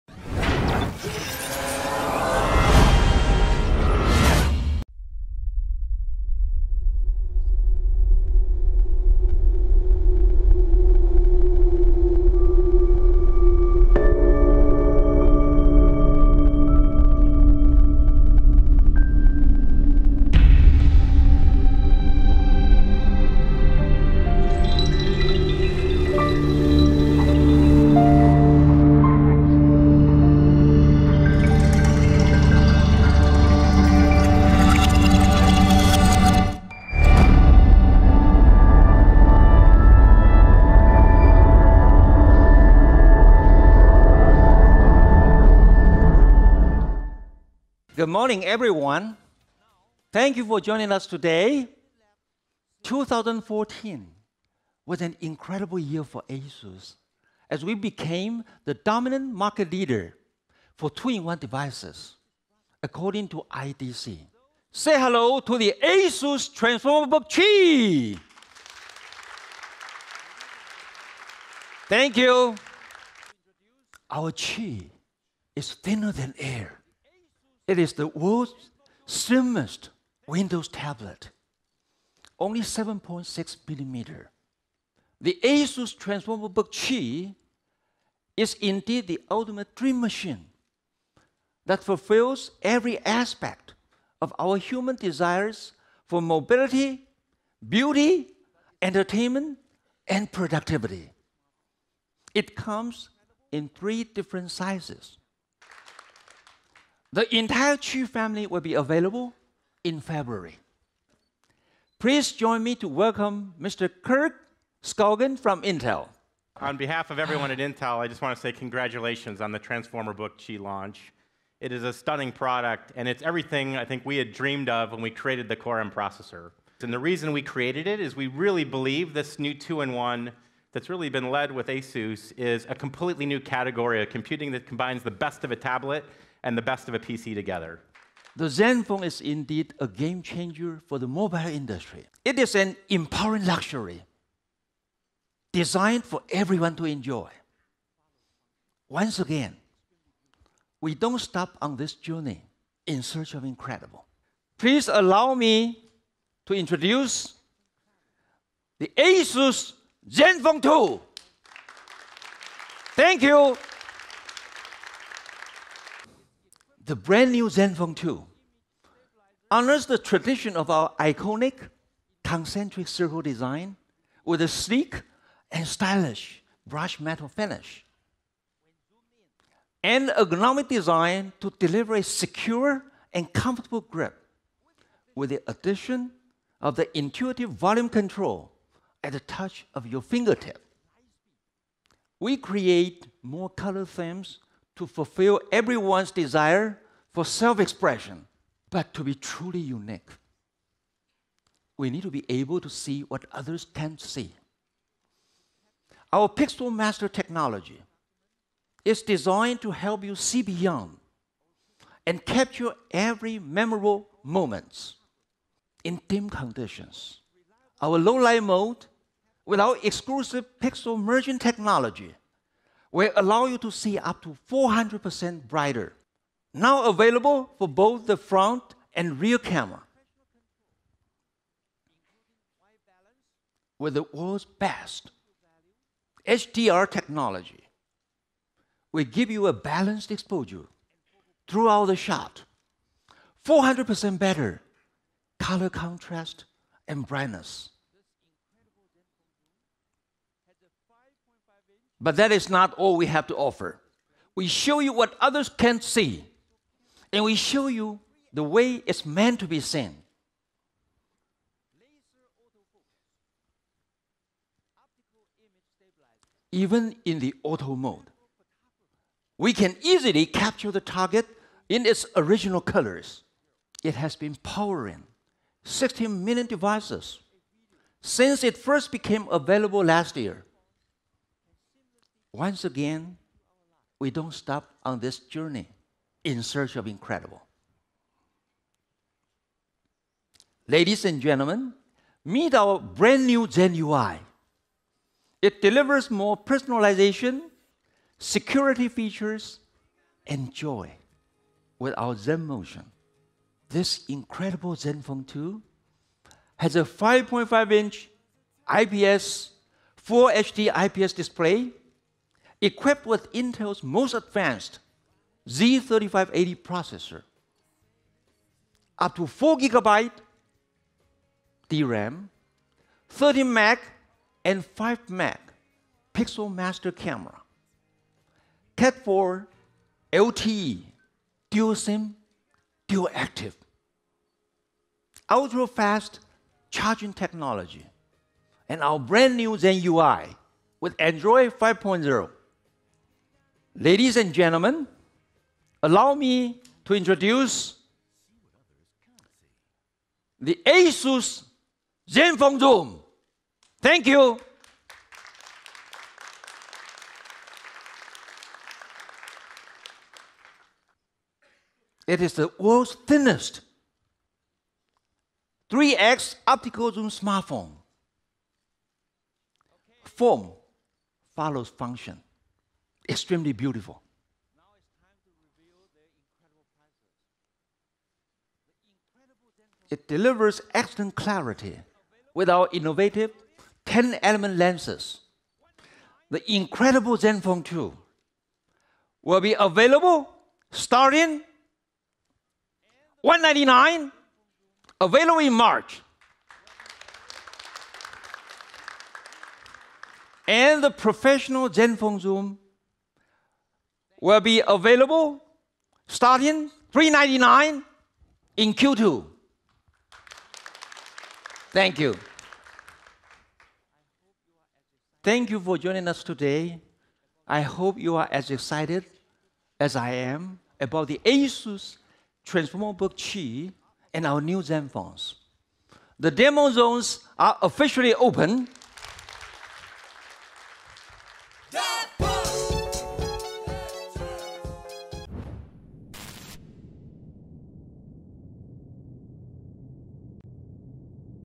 Experience 2Morrow: ASUS Press Event at CES 2015
ASUS Chairman Jonney Shih unveiled the company’s latest innovations during today’s press event at the 2015 International CES®. Mr. Shih introduced the full lineup of the groundbreaking ASUS Transformer Book Chi Series — powerful, razor-thin 2-in-1 laptops that transform instantly into the world’s slimmest Windows tablets.Mr. Shih then unveiled the much-anticipated ZenFone 2, which features a refined ergonomic body, advanced PixelMaster camera technology, the latest Intel® Atom™ Z3580 processor, and the brand new version of ASUS ZenUI that is designed for ultimate personalization.